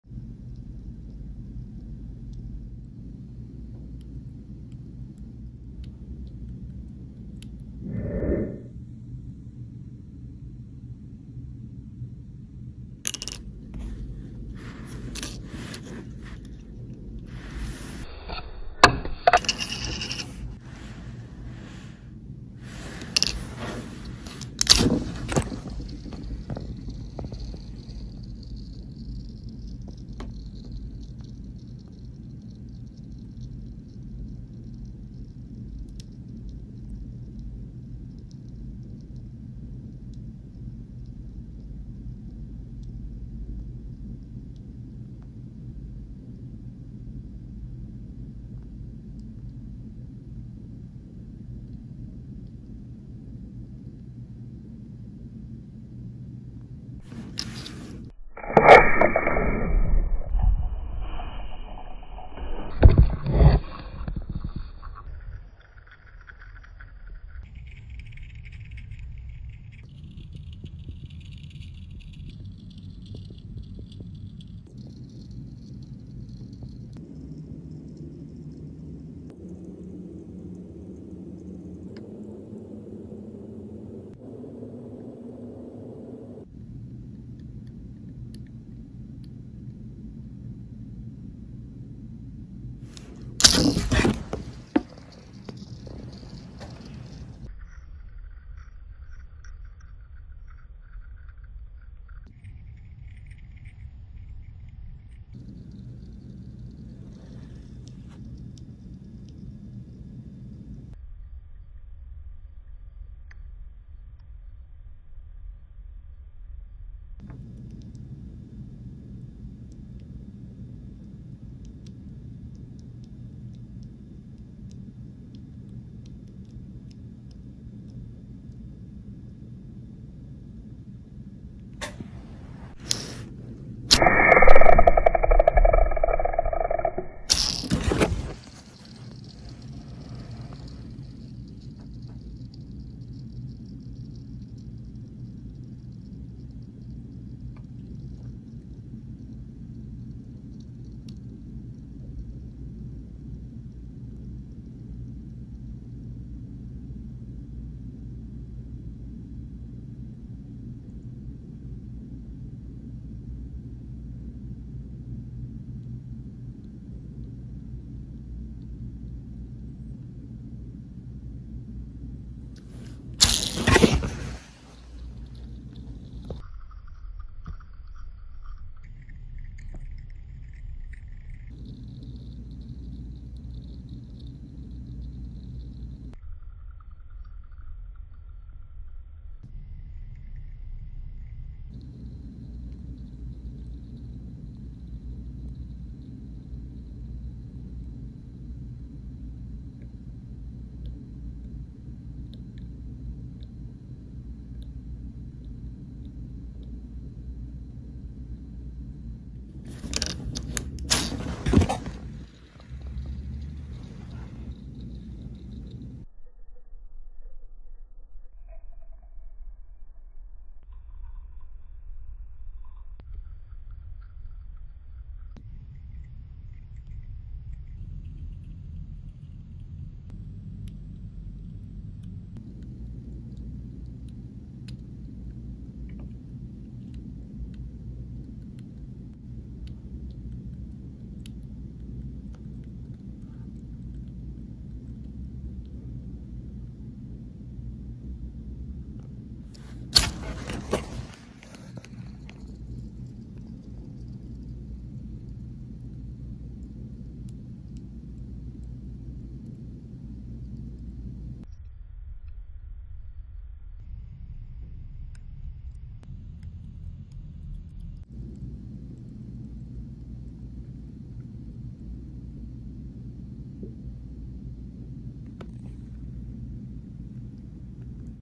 ASMR - Spinning Picture of Ed Sheeran’s Cat and Elton John’s Hand on Bottle Cap
ASMR - Spinning pic of Ed Sheeran’s can held by Elton John on a bottle cap played at 500% sound random intervals in slow motion.